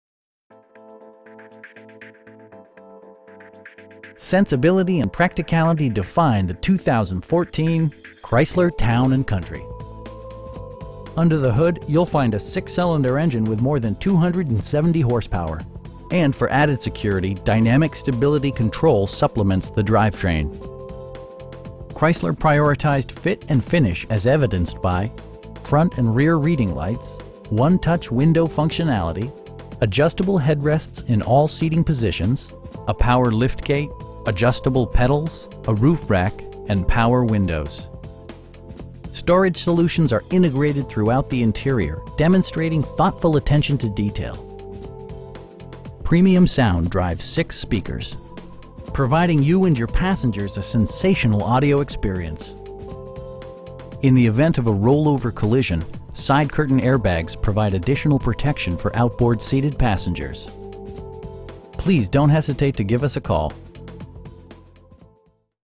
2014 Chrysler Town & Country in Coquitlam, BC V3E 1K9
2595 Barnett Highway.

Sensibility and practicality define the 2014 Chrysler Town & Country. Under the hood you'll find a 6 cylinder engine with more than 270 horsepower, and for added security, dynamic Stability Control supplements the drivetrain. Chrysler prioritized fit and finish as evidenced by: front and rear reading lights, 1-touch window functionality, adjustable headrests in all seating positions, a power liftgate, adjustable pedals, a roof rack, and power windows. Storage solutions are integrated throughout the interior, demonstrating thoughtful attention to detail. Premium sound drives 6 speakers, providing you and your passengers a sensational audio experience. In the event of a rollover collision, side curtain airbags provide additional protection for outboard seated passengers. You will have a pleasant shopping experience that is fun, informative, and never high pressured. Please don't hesitate to give us a call.